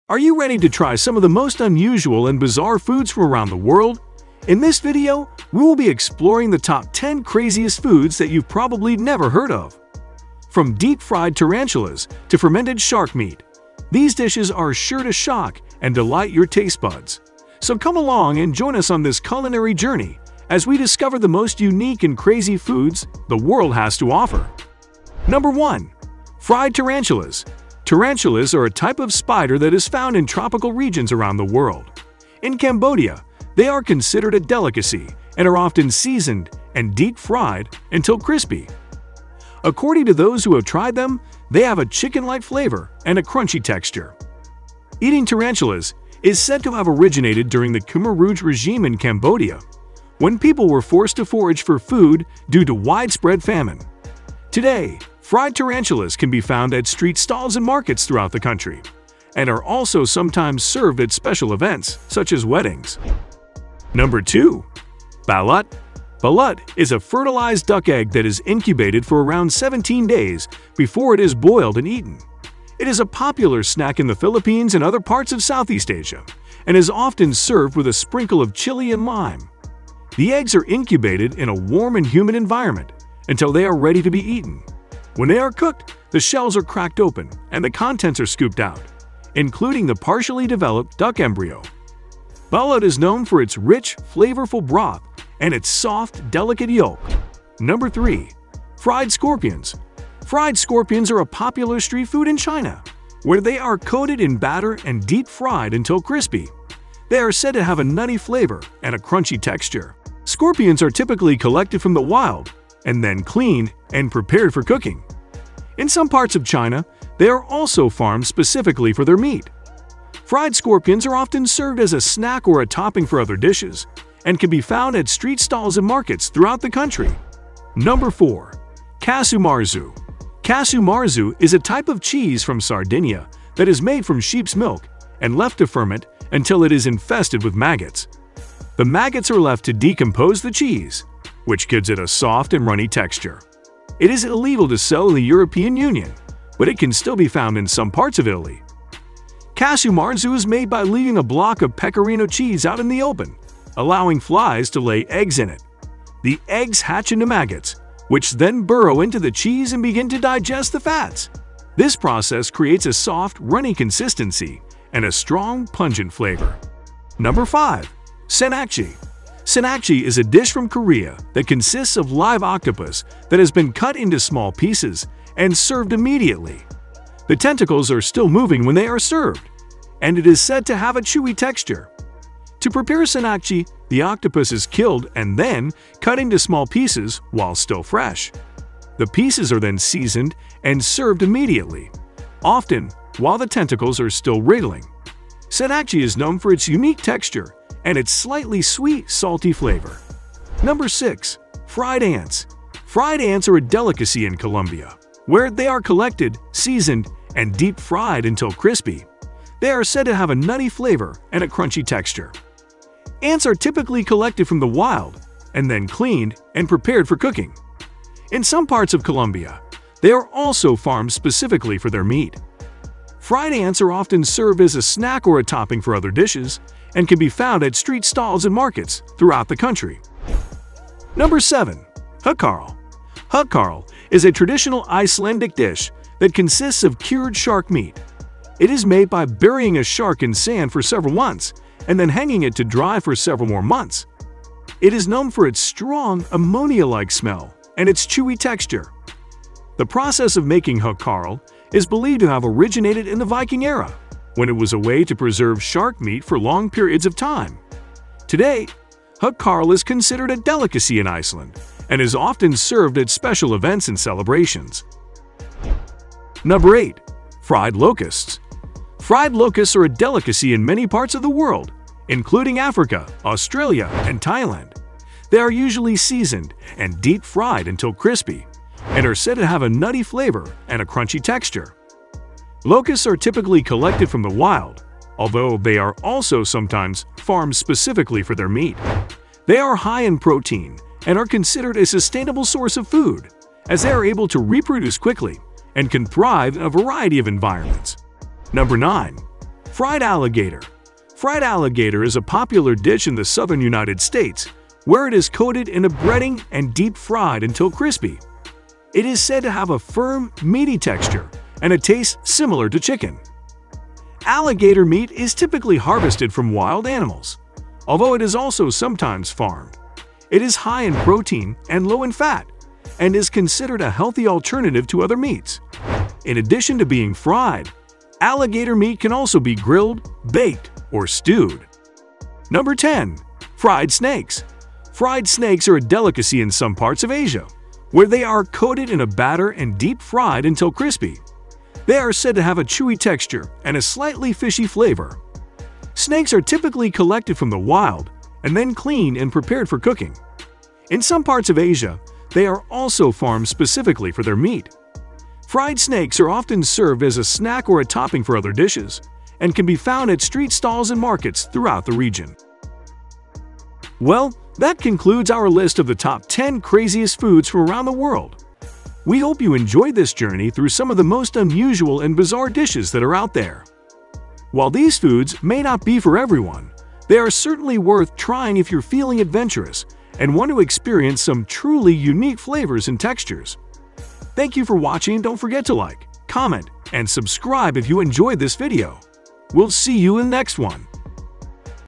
Top 10 Most Shocking Foods in the World: Would You Dare to Try Them?
Are you a foodie looking for a wild culinary adventure? Look no further! In this video, we count down the top 10 most craziest foods from around the globe. From fermented shark meat in Iceland to live octopus in South Korea, you won't believe some of the bizarre delicacies that people enjoy. Watch until the end to see the number one spot on our list.